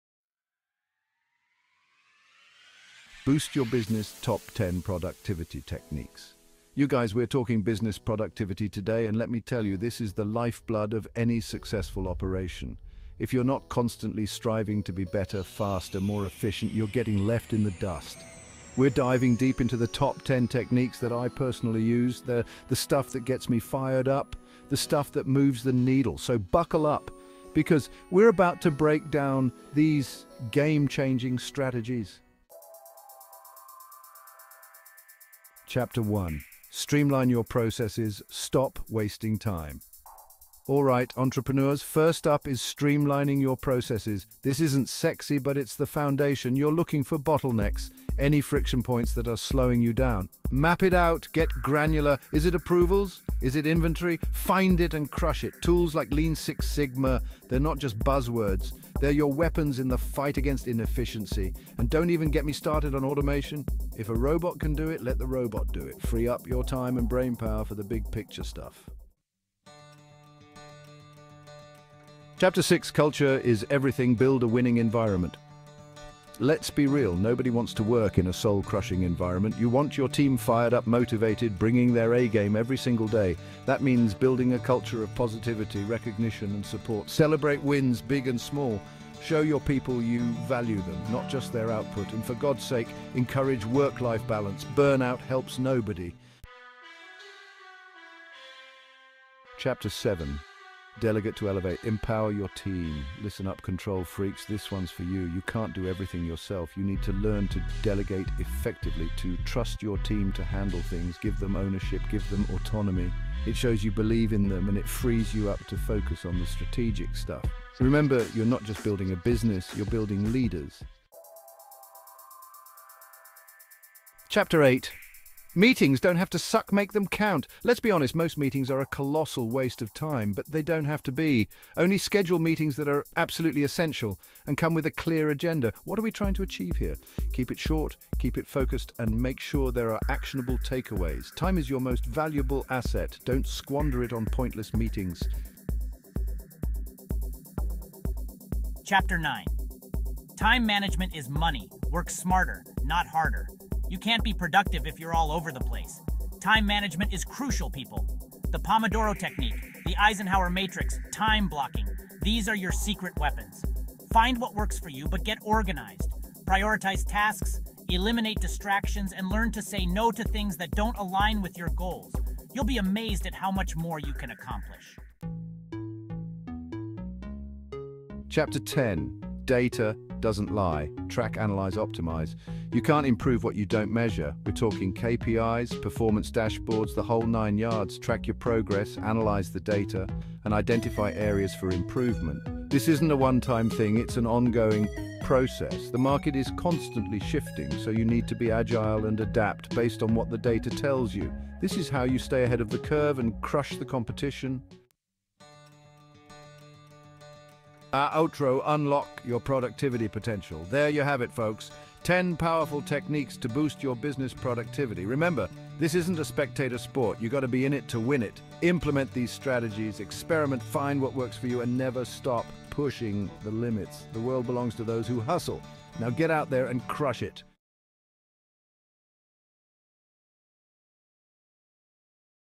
Unleash Your Business Potential: Top 10 Cutting-Edge Strategies for Maximizing Productivity
Welcome to our latest video, where we dive deep into the top 10 cutting-edge strategies to maximize productivity and boost your business to new heights! 🚀

In today's fast-paced world, staying ahead of the competition requires more than just hard work. You need smart, innovative approaches to streamline processes, leverage technology, and empower your team. Whether you're a small business owner or a corporate leader, these strategies are designed to help you achieve optimal performance and sustainable growth.

🔹 Streamline Processes: Discover how to identify and eliminate bottlenecks in your workflows for smoother operations.
🔹 Invest in Technology: Learn about the latest tech tools that can enhance your productivity and efficiency.
🔹 Employee Training and Development: Find out how continuous learning can keep your team ahead of the curve.
🔹 Set Clear Goals and Objectives: Establish SMART goals to provide direction and motivation for your team.
🔹 Enhance Communication: Explore tools and techniques to improve communication and collaboration.
🔹 Foster a Positive Work Environment: Create a workspace that boosts morale and productivity.
🔹 Delegate and Empower Employees: Understand the power of delegation and employee autonomy.
🔹 Optimize Meetings: Transform your meetings into productive and actionable sessions.
🔹 Implement Time Management Techniques: Master time management methods to prioritize tasks effectively.
🔹 Monitor and Analyze Performance: Use KPIs and performance dashboards to track progress and make timely adjustments.

📌 Chapters:
00:00 - Introduction
01:20 - Streamline Processes
03:10 - Invest in Technology
05:00 - Employee Training and Development
06:40 - Set Clear Goals and Objectives
08:20 - Enhance Communication
10:00 - Foster a Positive Work Environment
11:40 - Delegate and Empower Employees
13:20 - Optimize Meetings
15:00 - Implement Time Management Techniques
16:40 - Monitor and Analyze Performance
18:30 - Conclusion

Join us on this journey to unlock your business's full potential. Let's get started!